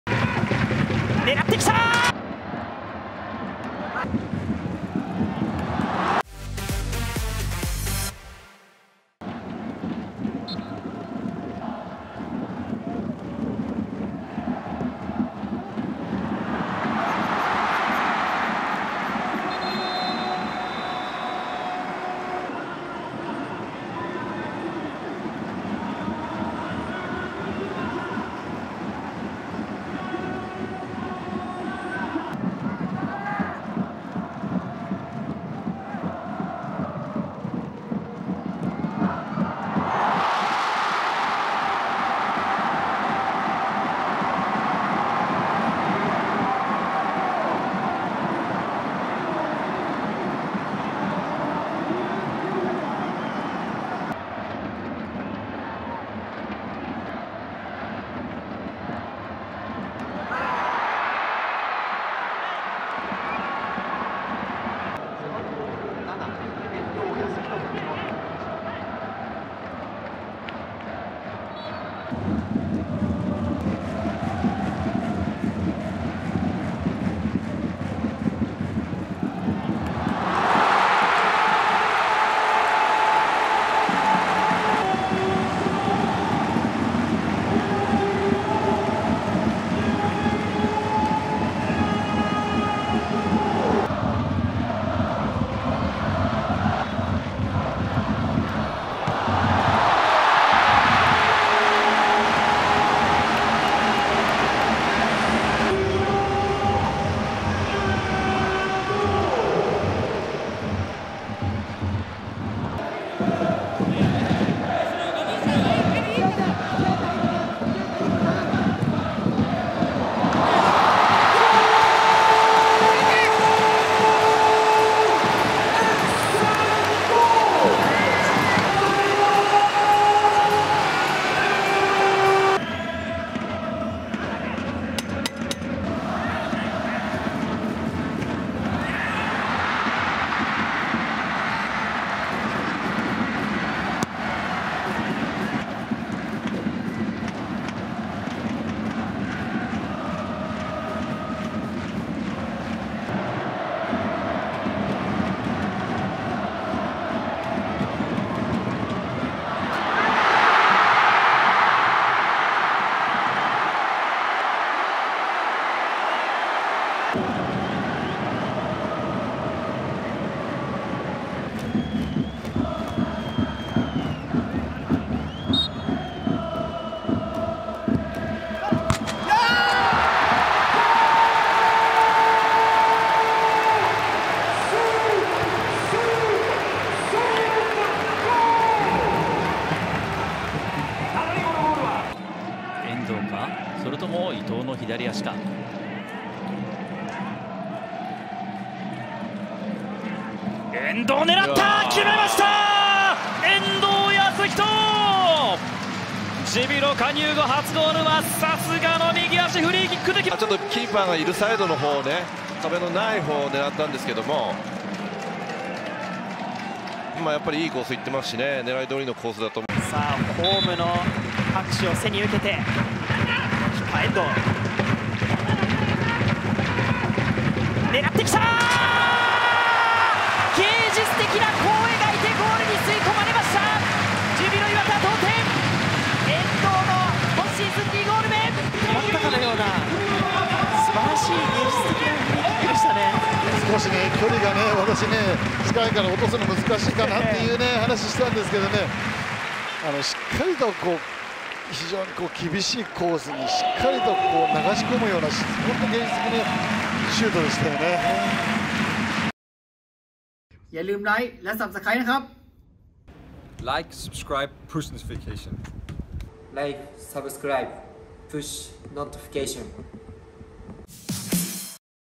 Yasuhito Endo's All FREE-KICK GOALS
Today, we’re throwing it back to all free-kick goals scored by Yasuhito Endo. He also bagged one so far this season for Júbilo Iwata. Which free-kick goal by Endo is still in ingrained in your memory? Let us know below!